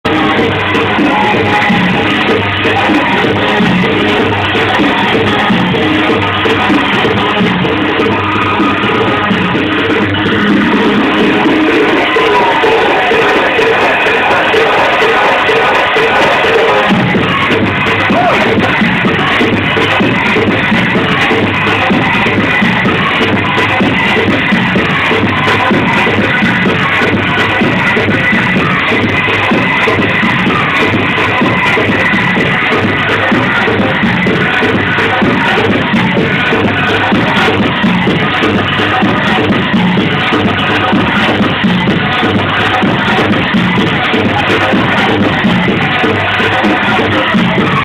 Delete aka Sergio Munoz @ D'Club
Delete aka Sergio Munoz @ D'Club on 12/28/08 in El Paso, TX.